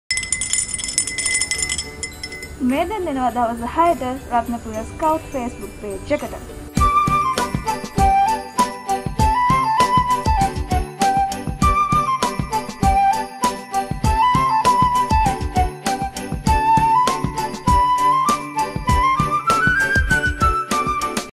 2020 12 28 සාම උදානය නත්තල් කැරොල් ගී මියැසිය
සාම උදානය නත්තල් කැරොල් ගී මියැසිය
රත්නපුර බාලදක්ෂ දිසාවේ සාමයේ පණිවිඩකරු සහ දිසා බාලදක්ෂ මාධ්‍ය ඒකකය සංවිධානය කරන "සාම උදානය නත්තල් කැරොල් ගී මියැසිය "